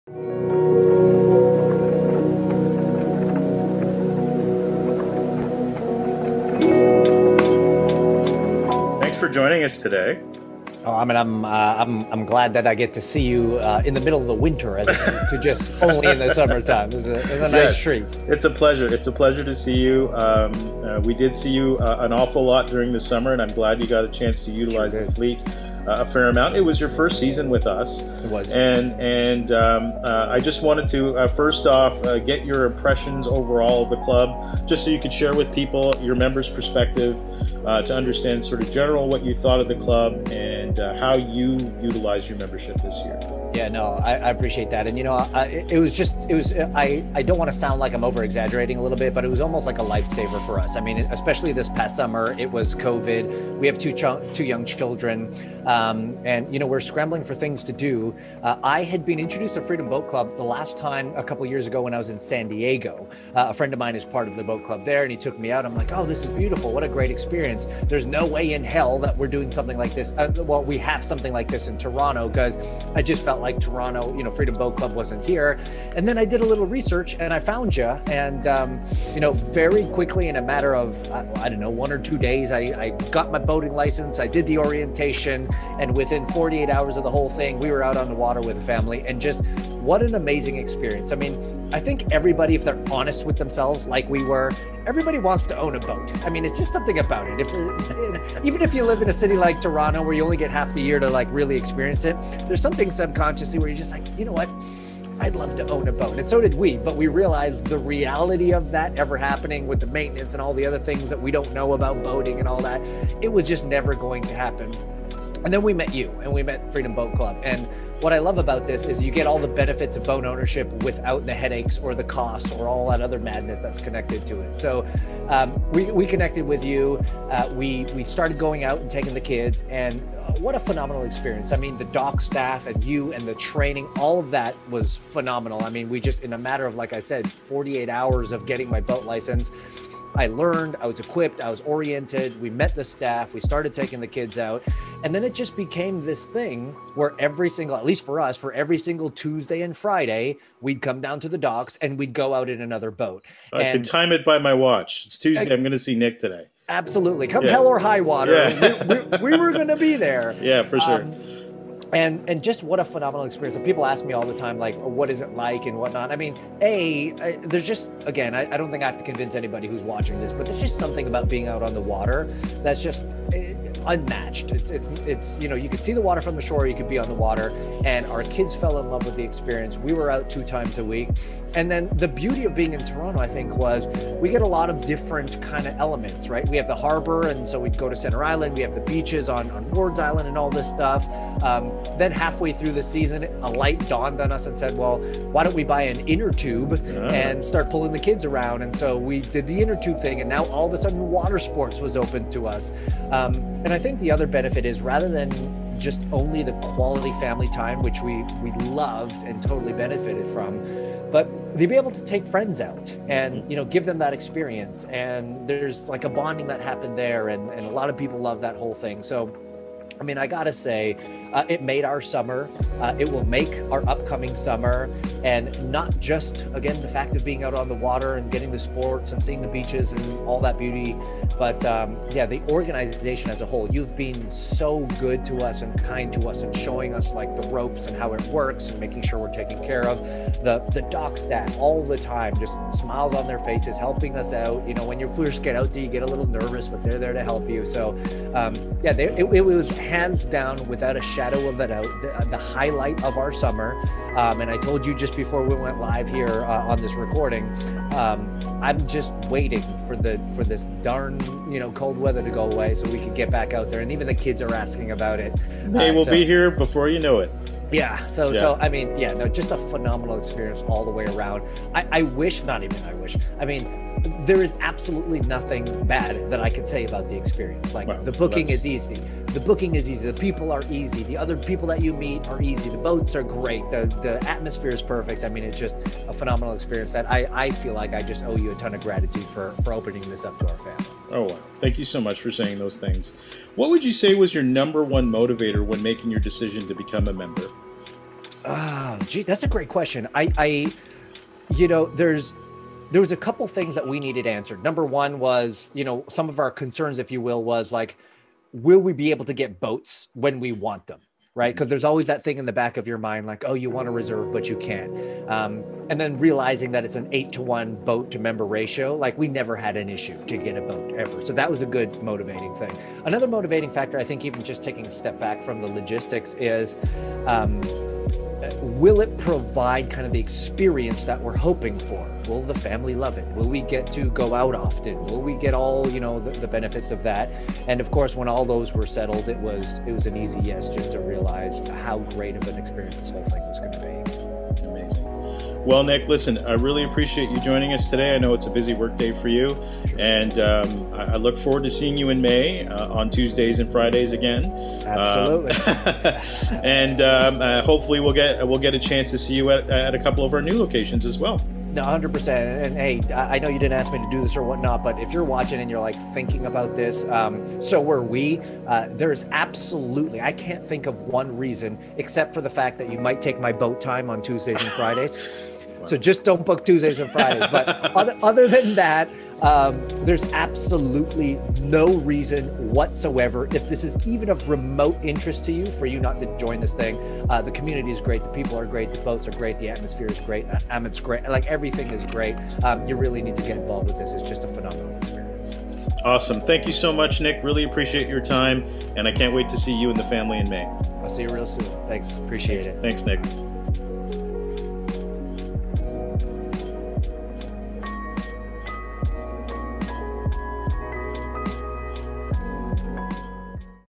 FBC Toronto Member Testimonial
Nic Kusmich joined us Live on Zoom at the Virtual o edition of  TIBS 2021!  Don’t take it from us!  Take it from Nic!